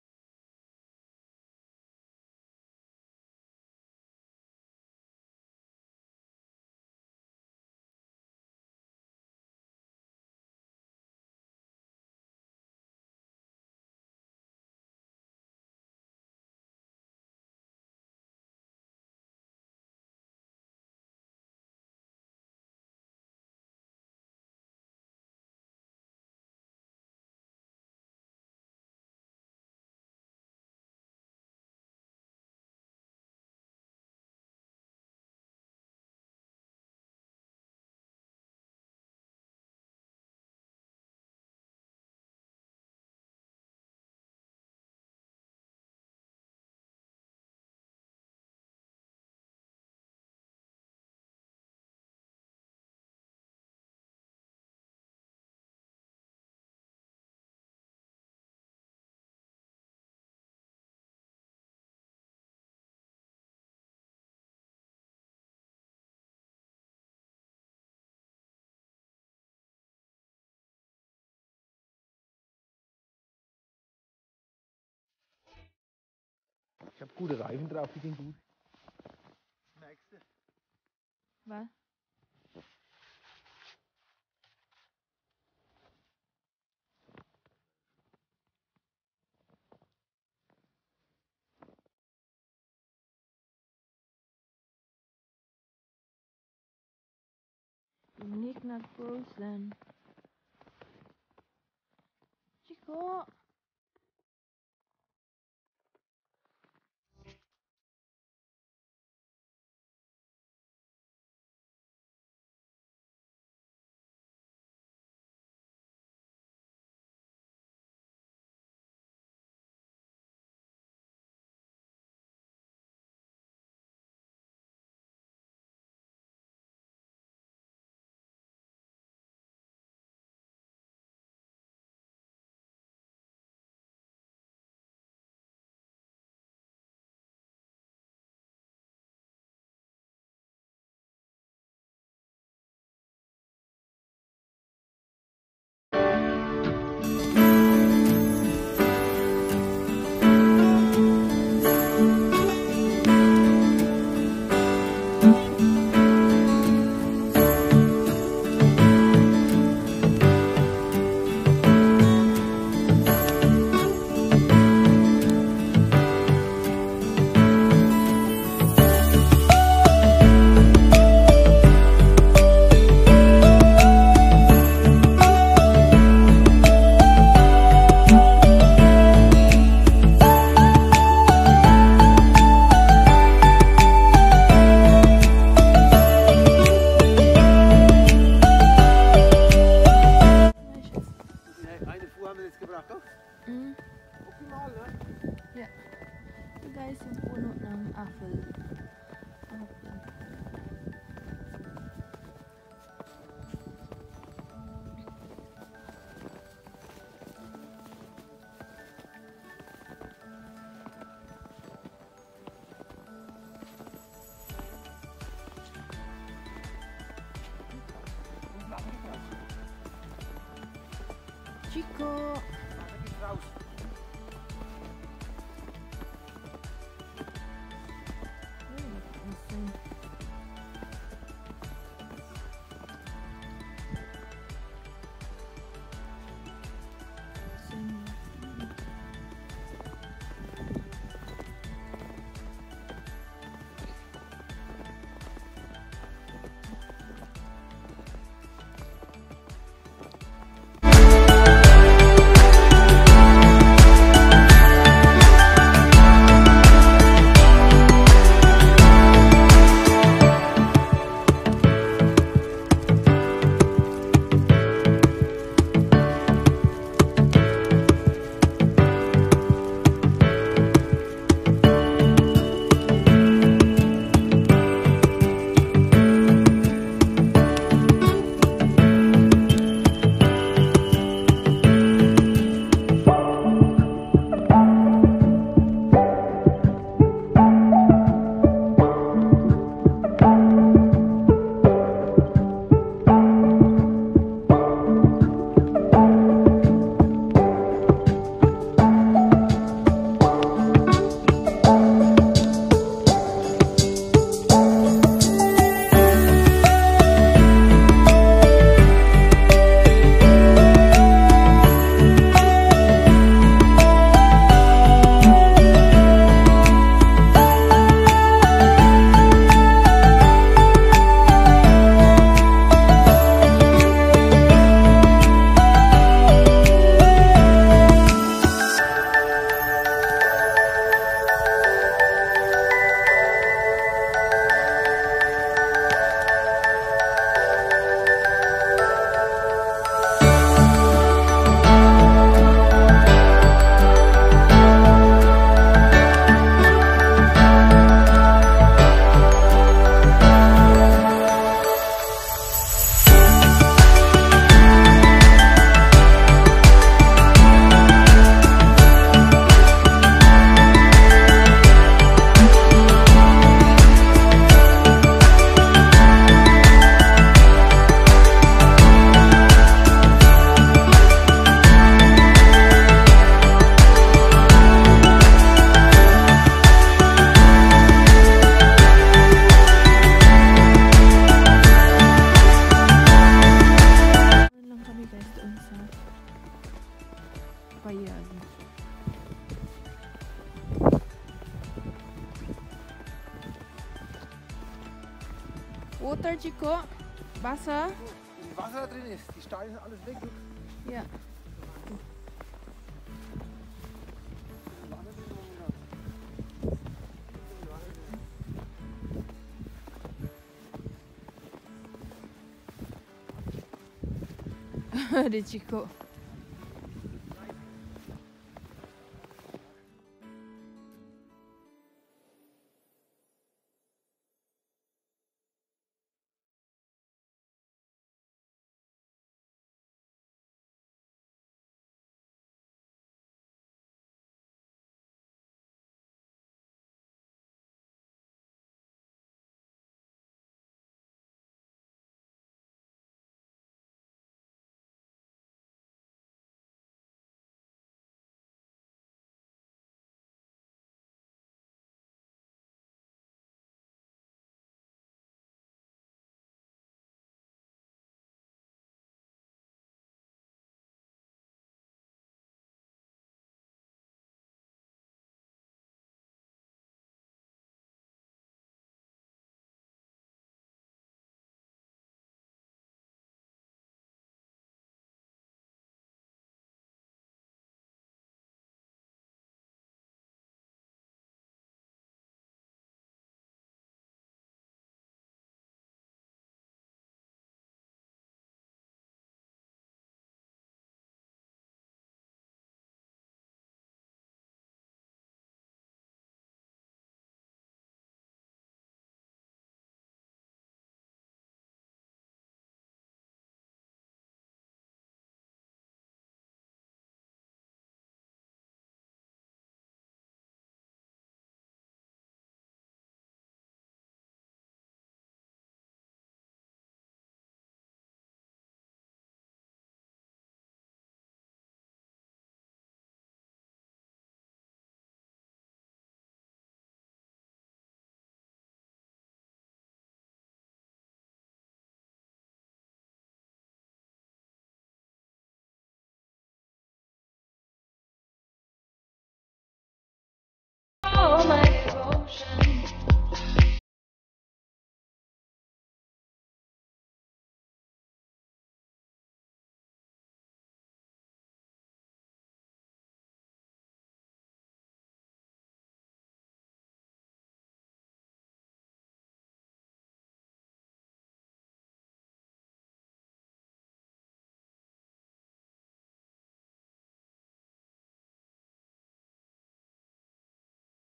Winter checking at the Lake||Walking with our Chicco(Border Collie Dog)
Stay safe everyone mwaaahhh💋

Liebe Grüße
Doroshi chan